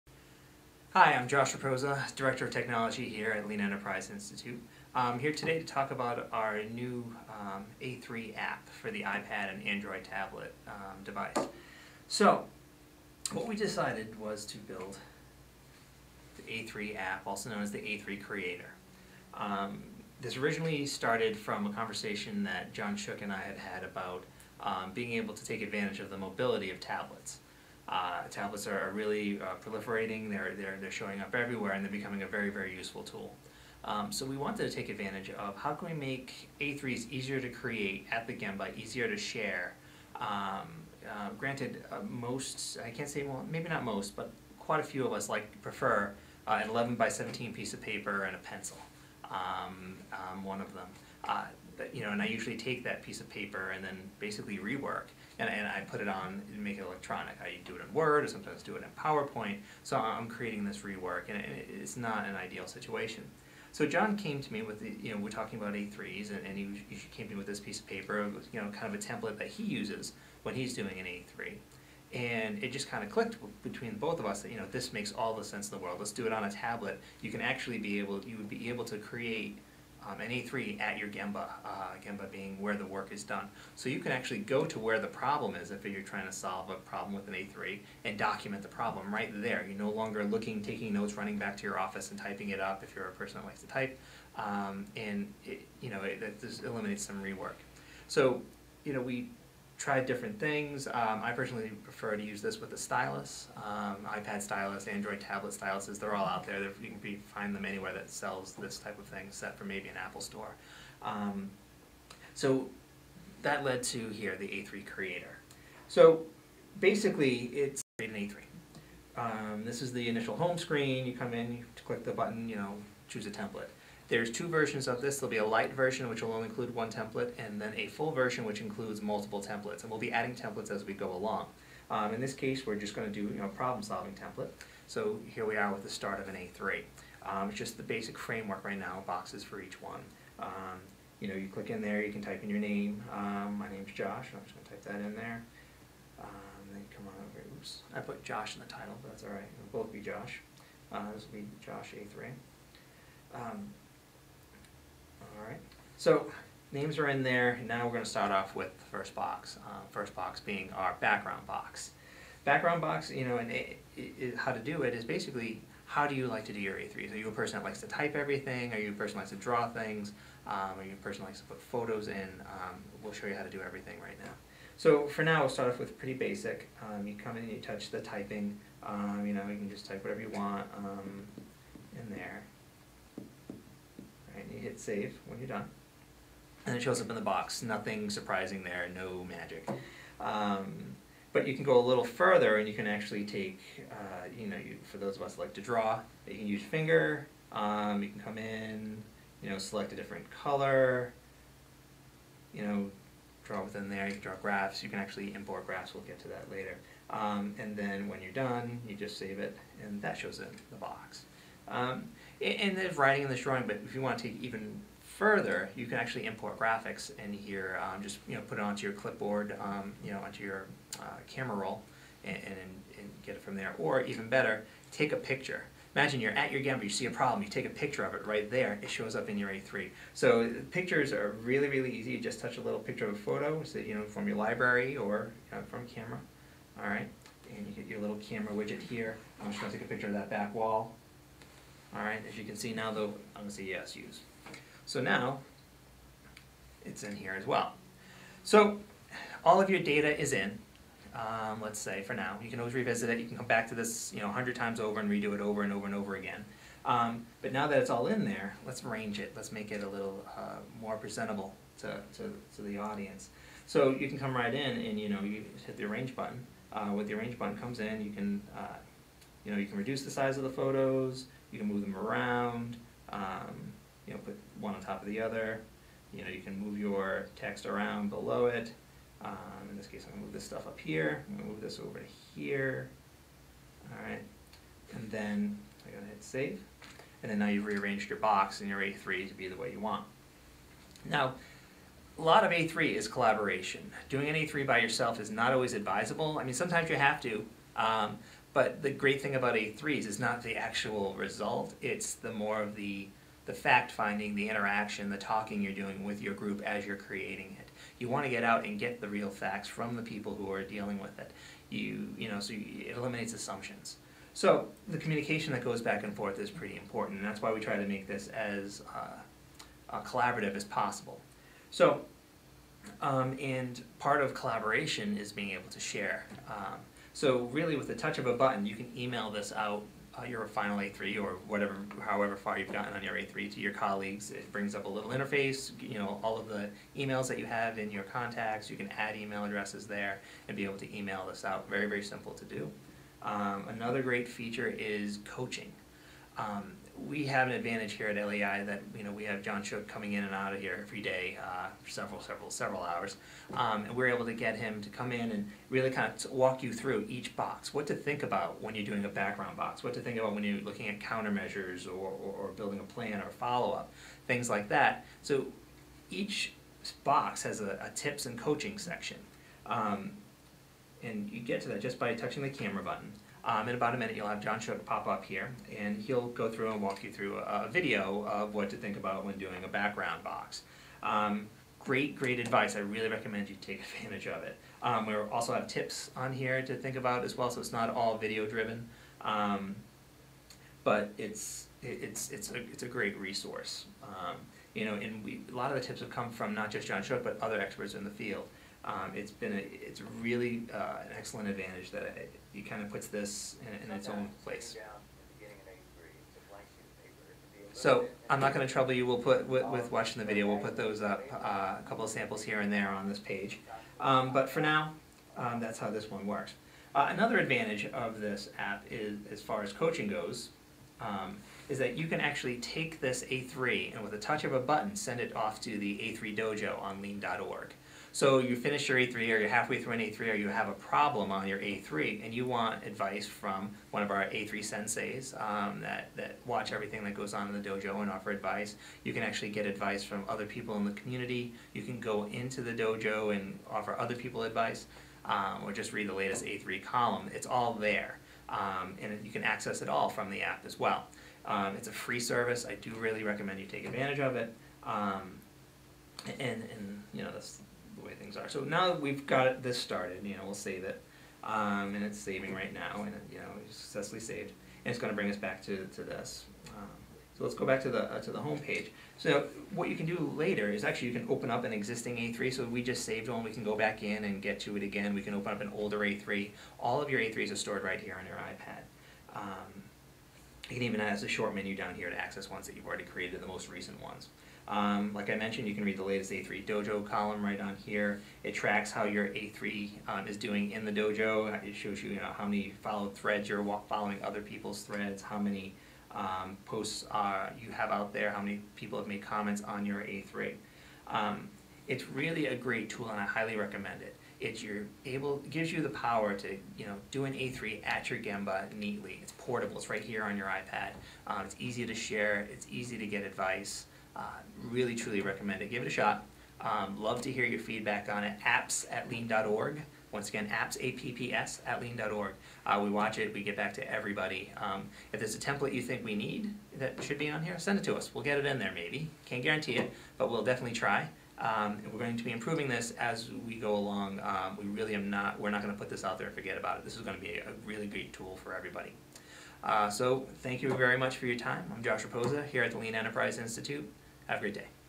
The A3 Creator App for iPads and Android Tablets Demonstrated
In this video Joshua Rapoza walks you through the A3 Creator app created by the Lean Enterprise Institute. This app helps you create A3s for problem solving, PDCA, and Strategy Development. A3 think plays a major role in lean management.